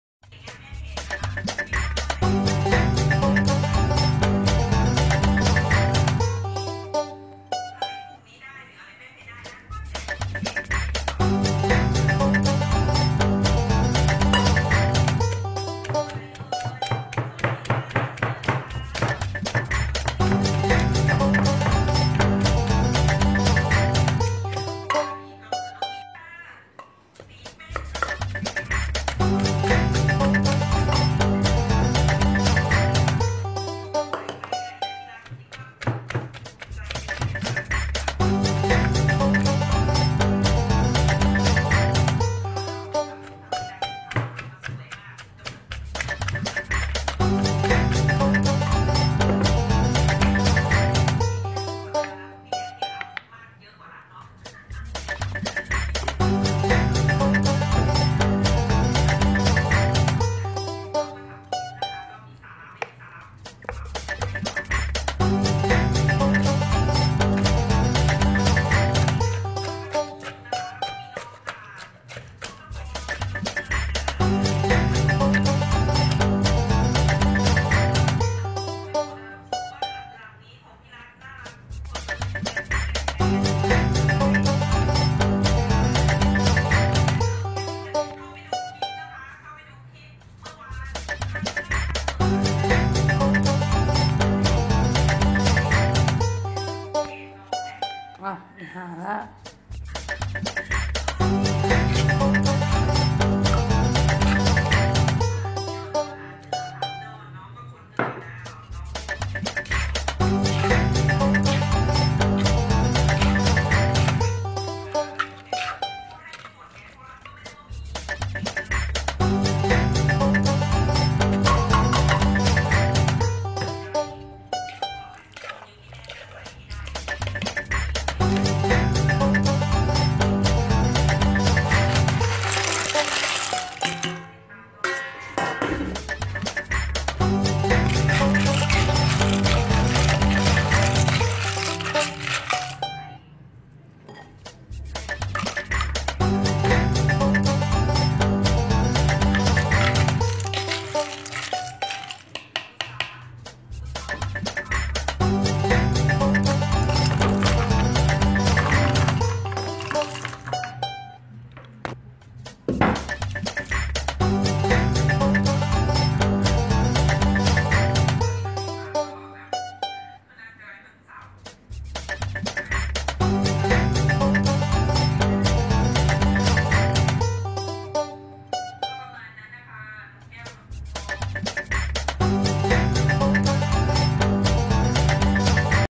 # ตำหอยแครง
ลวกหอย 20 วิน๊อคน้ำเย็นก่อนแล้วเอามาตำหอยจะเด้งมากมาก 😁😅